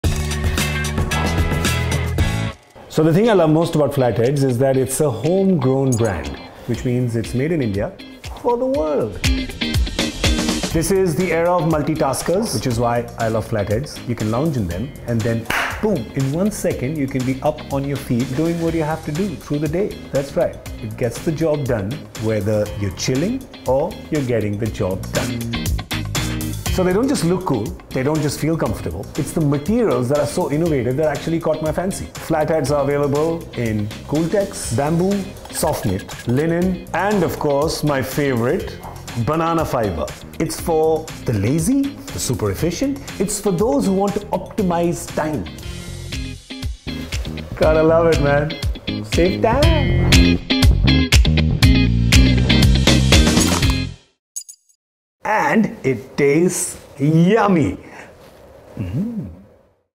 GAURAV KAPUR I FLATHEADS
Meet the greatest shoe-man ever. After all, there's no business like shoe business 😎 Gaurav Kapur joined our journey in building the most innovative, responsible and relevant footwear for the urban workforce and we couldn't be more thrilled!